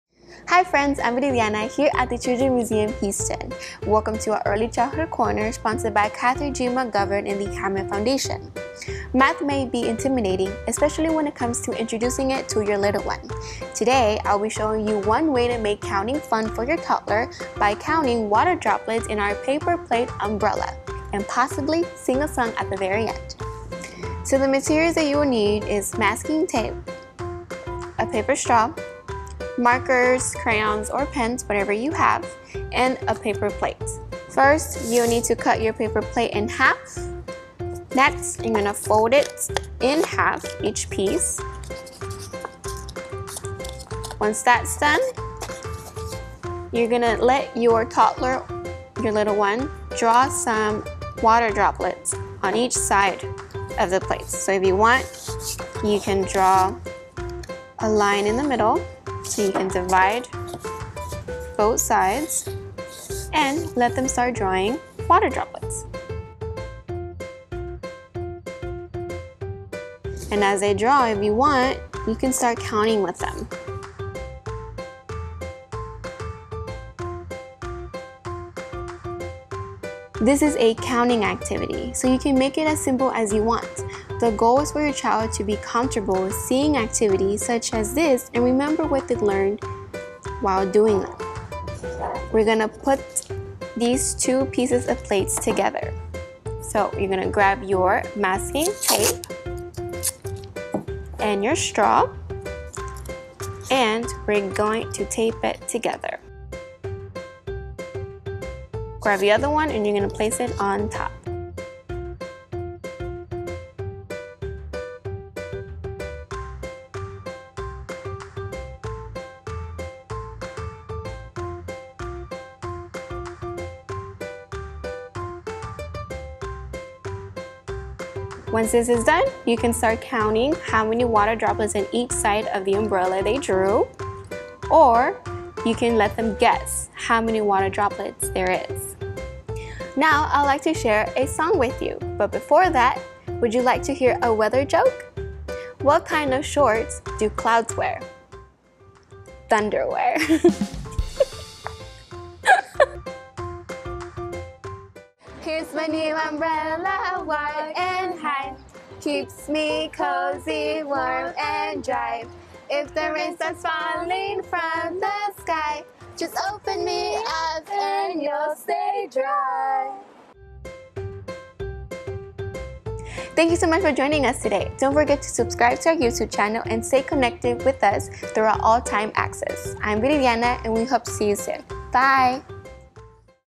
Umbrella Story - Early Childhood Corner
Follow Children's Museum Houston educator, Viri, and make counting fun for your toddler by counting water droplets on a paper plate umbrella.

This Early Childhood Corner episode is part of our "All-Time Access" program to connect you with our educators wherever you are - at school, at home, at play!  Explore for FREE today